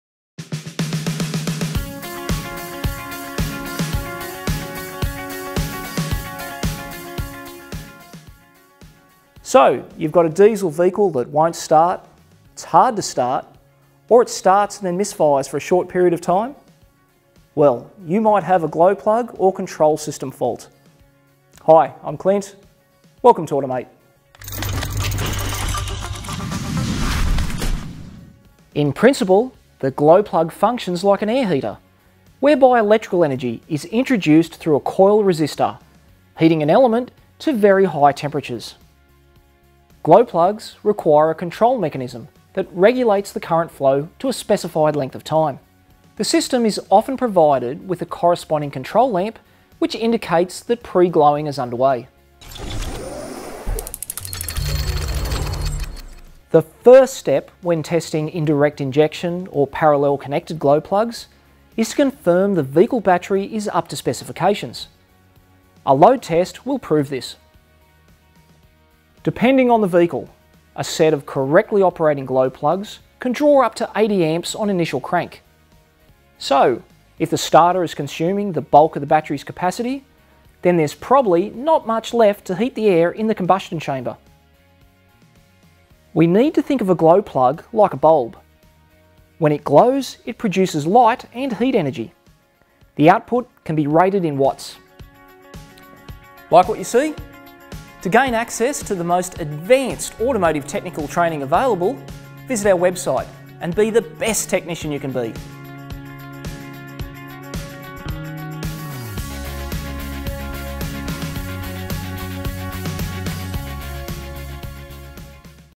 Indirect Glow Plug System - Testing Procedures
Have you been looking for a better way to test and diagnose glow plug or glow plug control system faults?

Become a member today to watch the full training video to discover how indirect or parallel connected glow plugs and their control system operate, and which tools should be used and what checks must be performed when confronted with a fault.